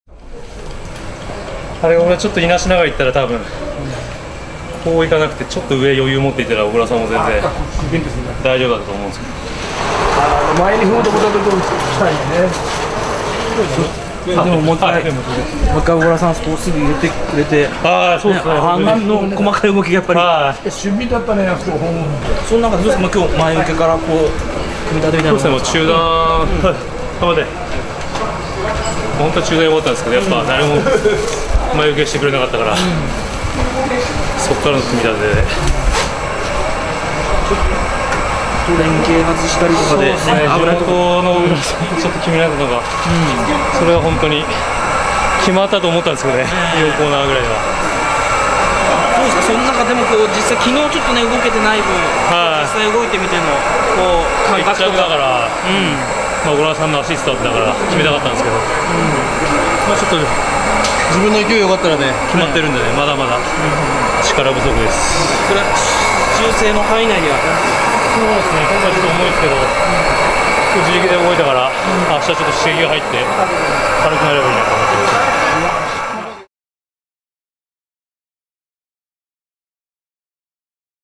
【小松島競輪ＧⅢ開設６７周年記念】井上昌己が勝利も喜びは半減
【小松島競輪ＧⅢ阿波おどり杯争覇戦(７日）】井上昌己（37）が懸命に仕掛けて１着をゲット。番手の小倉竜二とワンツーかと思われたが、直線で小倉が落車するアクシデントが発生したため喜びも控えめだ。